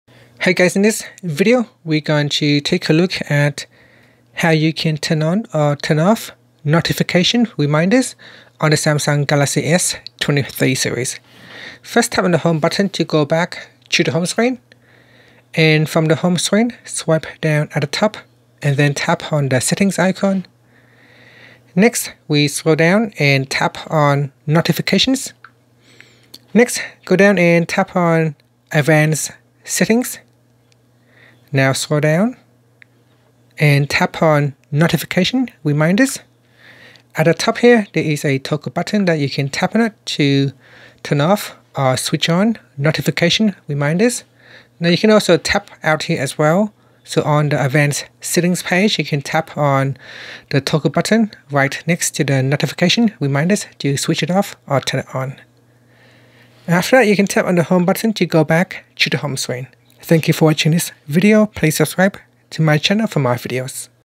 Galaxy S23's: How to Turn On/Off Notifications Reminders
Learn how you can turn on or off notification reminders on the Samsung Galaxy S23.

In this tutorial video, you will learn how to turn on or off notification reminders on your Samsung Galaxy S23. Notification reminders can be a helpful feature if you tend to miss important notifications, as it will remind you of unread notifications at set intervals. However, if you find the reminders to be more of a distraction than a help, you may want to turn them off. The video will guide you step-by-step through the process of enabling or disabling notification reminders on your device. By the end of this tutorial, you will have full control over your notification settings and be able to customize them to your liking.